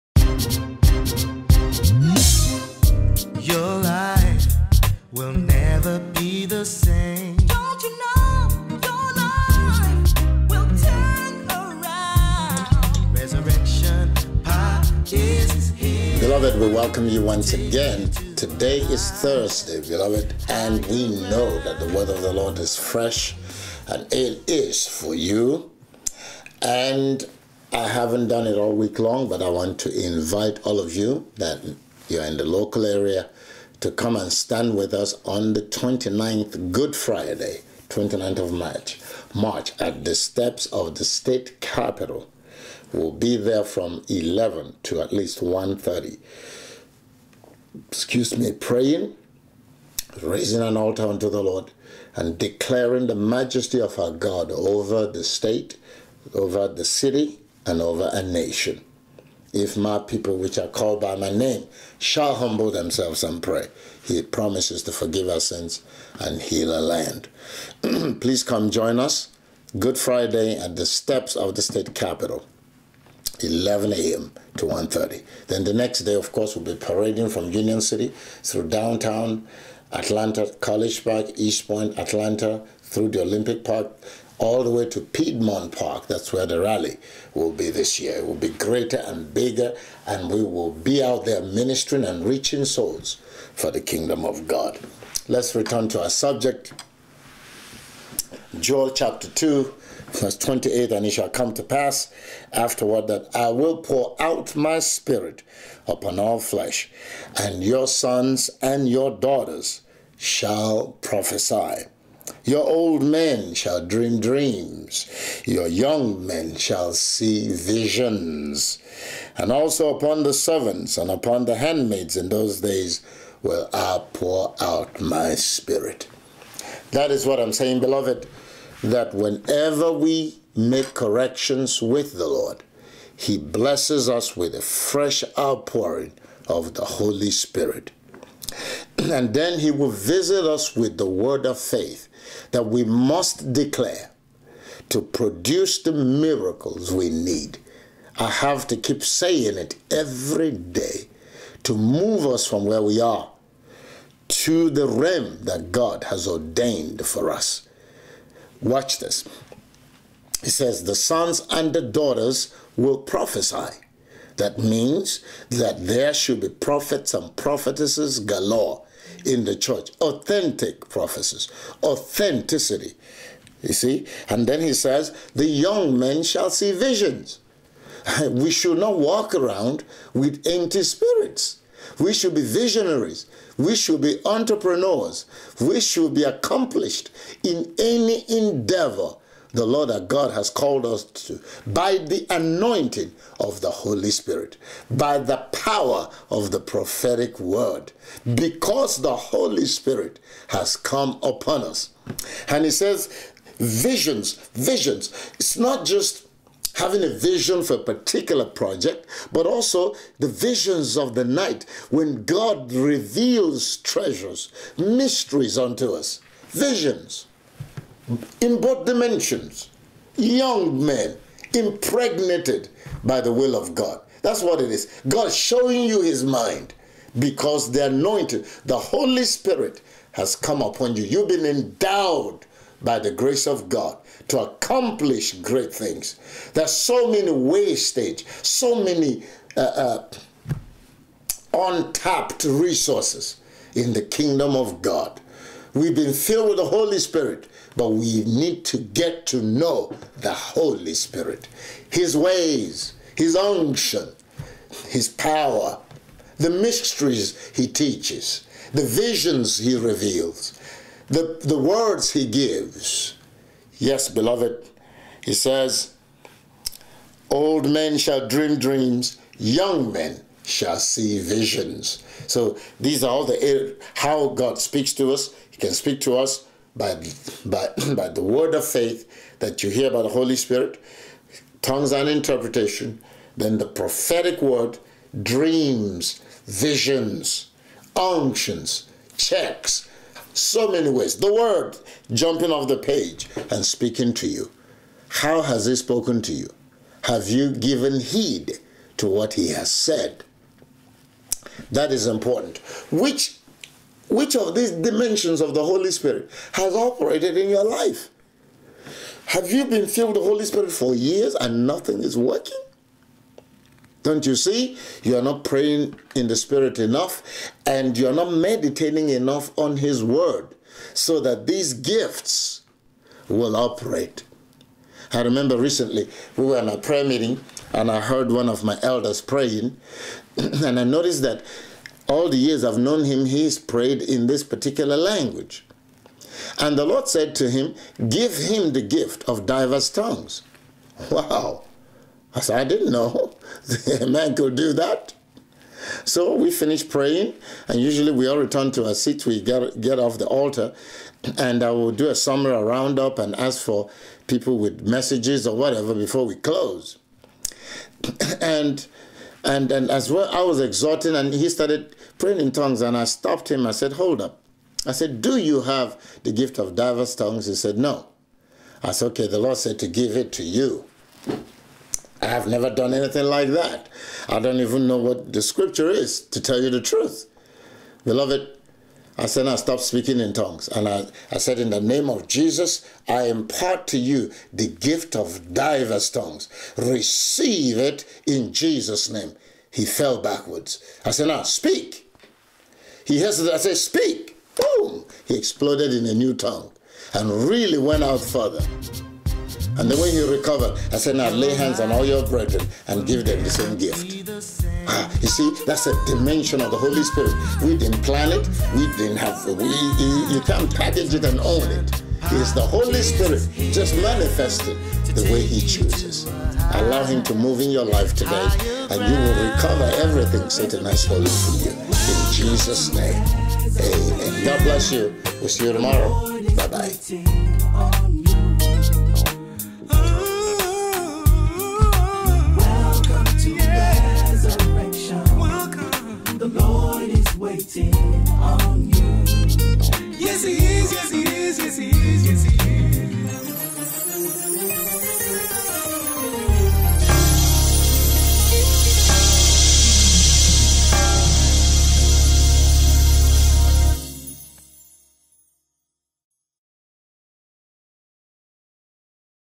2.28.13 -- The Power of Seed...The Dreamers & Visionaries
We should be visionaries. We should be entrepreneurs. We should be accomplished in any endeavor that the Lord our God has called us to do by the anointing of the Holy Spirit, by the power of the prophetic Word, because the Holy Spirit has come upon us. Listen to a Voice of Resurrection radio broadcast on Love 860 AM, Monday - Friday at 7:45am EST. Or Listen Live